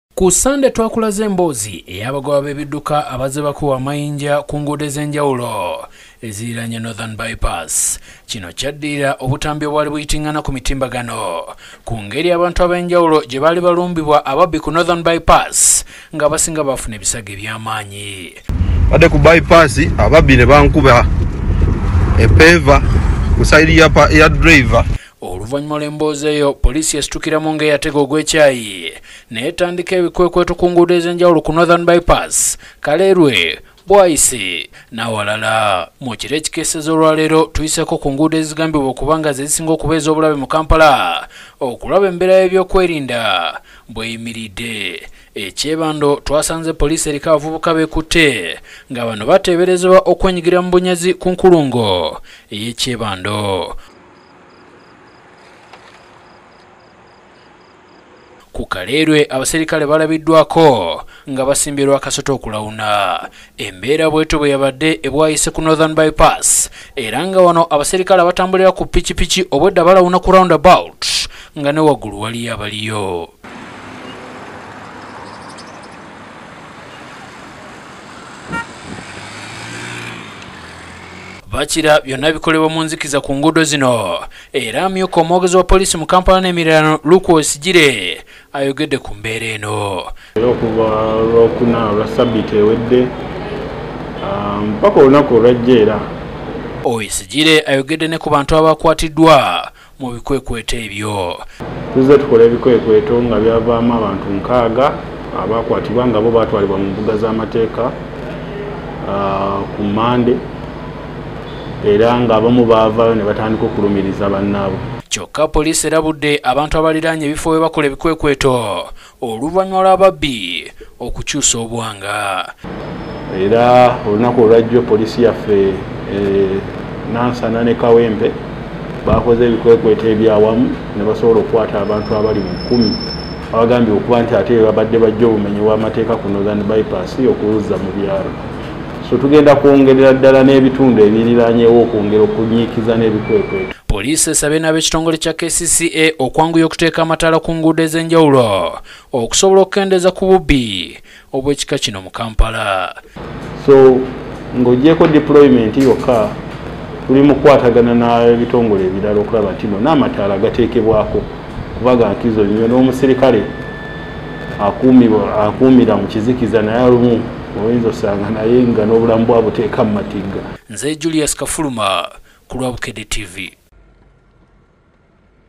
Agataliikonfuufu: Poliisi eyongedemu amaanyi mukulwanyisa obubbi ku nguudo nadala ku Northern Bypass
Oluvanyuma lw'okukulaga emboozi y'ababbi abakozesa amayiinja okubba ku Northern Bypass, Poliisi etandiise okukola ebikwekweto era erina abavubuka abasoba mu 20 beekute.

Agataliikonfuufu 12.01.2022
Julius Kafuluma